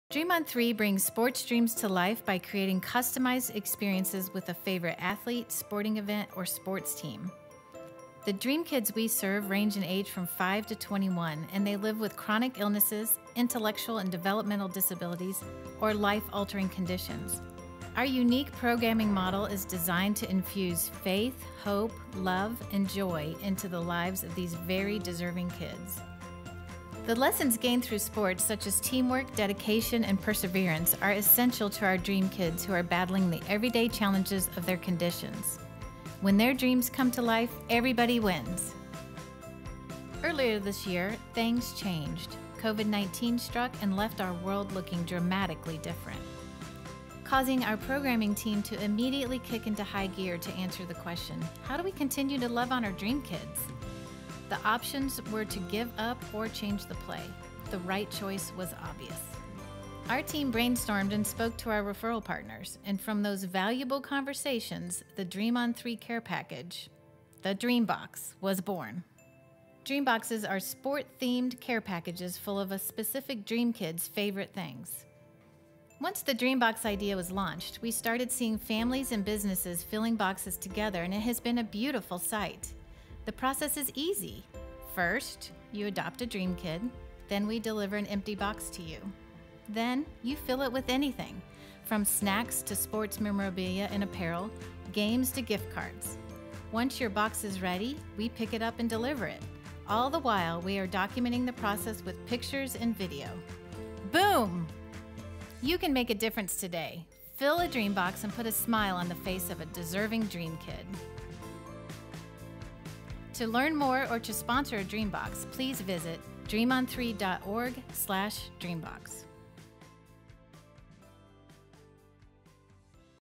The DreamBox Process
Dream On 3, a Charlotte, NC-based organization that makes SPORTS dreams come true for kids living with life-altering medical conditions and/or intellectual disabilities. Dream On 3 uses the unifying principles of sports and the power of teamwork to bring hope, love, and joy to dreamers, ages 5-21.

One of the ways Dream On 3 accomplishes this is through their DREAMBOX PROGRAM. Kids receiving medical treatment for chronic illnesses or kids who are spending their lives in and out of the hospital need something to make them smile... something to lift their spirits. This is what a DreamBox can do! These customized care packages are put together specifically for each individual Dream Kid and delivered to them – at their home or hospital.

Companies, families, dental offices, sports teams, and many other groups and organizations have pitched in to “adopt a Dream Kid” and fill a DreamBox. We would love for your group to be next.

Are you ready to adopt a Dream Kid and bring a smile to the face of a kid who really needs it?